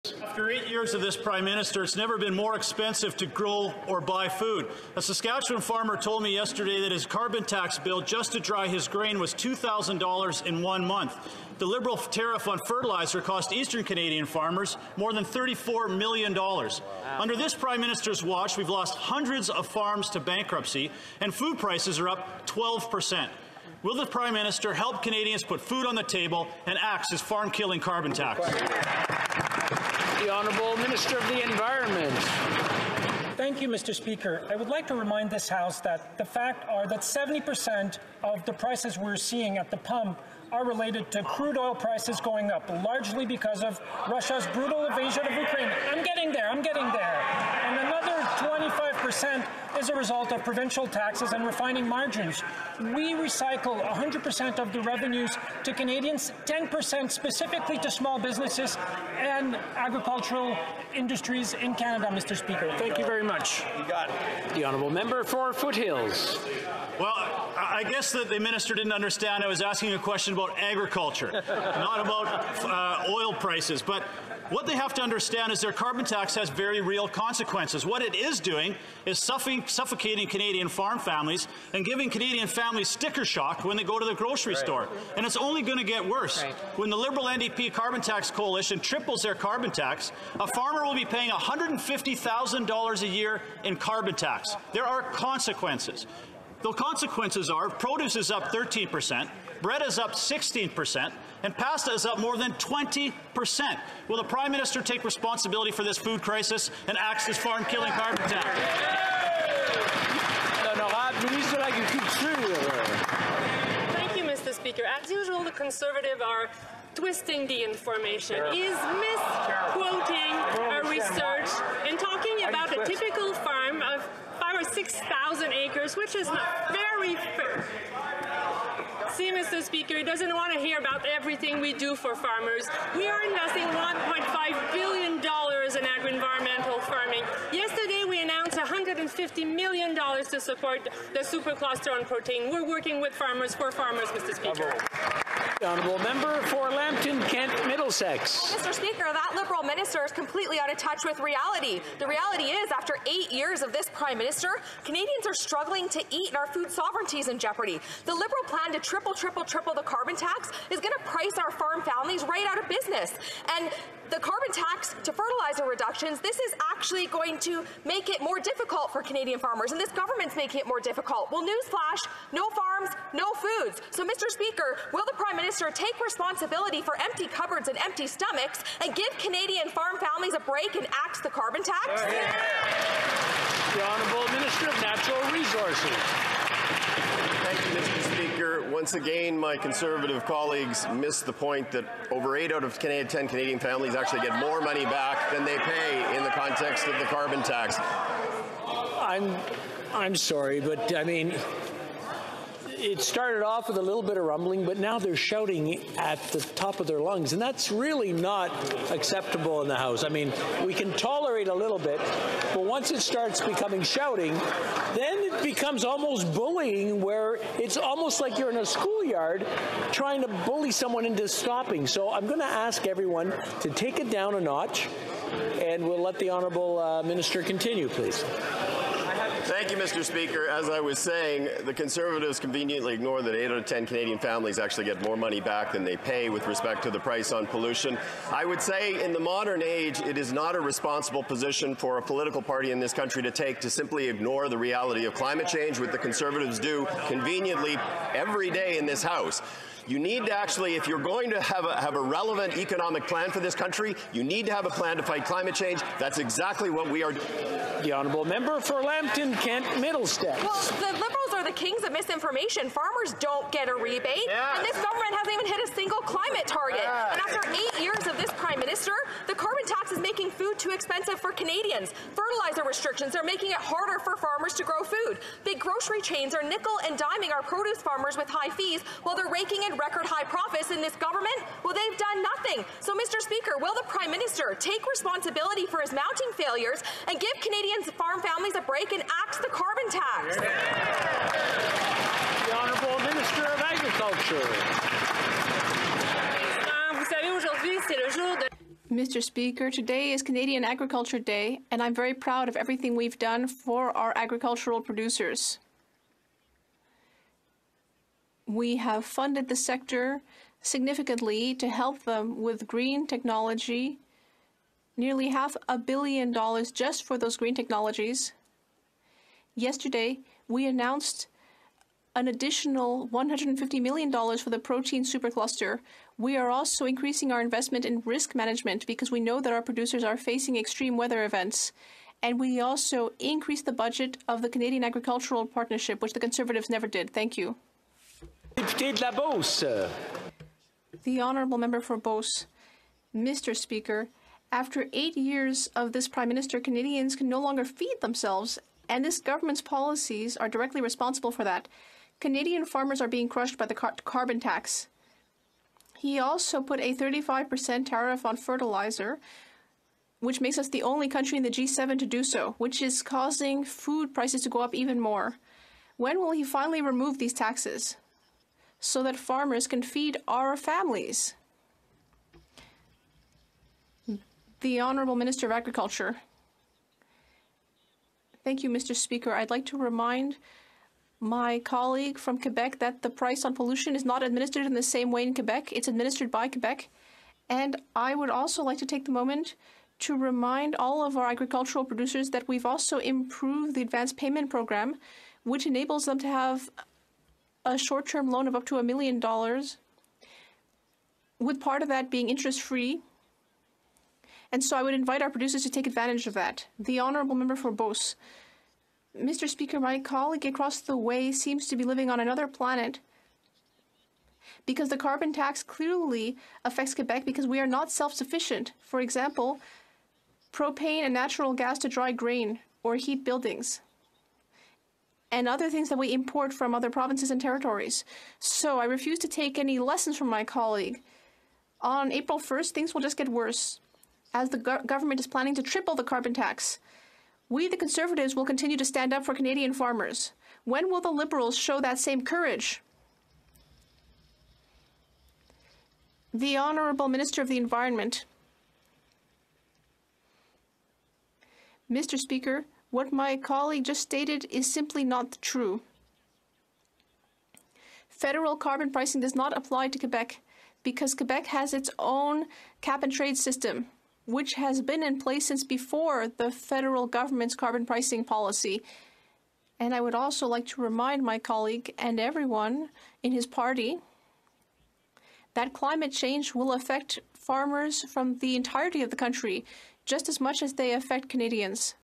Under Trudeau's watch, hundreds of farms went bankrupt, and Canadian food sovereignty is at risk
'After eight years of the Prime Minister, it has never been more expensive to grow or buy food.
    A Saskatchewan farmer told me yesterday that his carbon tax bill just to dry his grain was $2,000 in one month. The Liberal tariff on fertilizer costs eastern Canadian farmers more than $34 million. Under the Prime Minister's watch, we have lost hundreds of farms to bankruptcy, and food prices are up 12%.
    Will the Prime Minister help Canadians put food on the table and axe his farm-killing carbon tax?'
'That Liberal minister is completely out of touch with reality. The reality is that after eight years of the Prime Minister, Canadians are struggling to eat and our food sovereignty is in jeopardy.
    The Liberal plan to triple the carbon tax is going to price our farm families right out of business. The carbon tax and fertilizer reductions are going to make it more difficult for Canadian farmers. The government is making it more difficult. Here is a news flash: no farms, no food.
    Will the Prime Minister take responsibility for empty cupboards and empty stomachs, give Canadian farm families a break and axe the carbon tax?'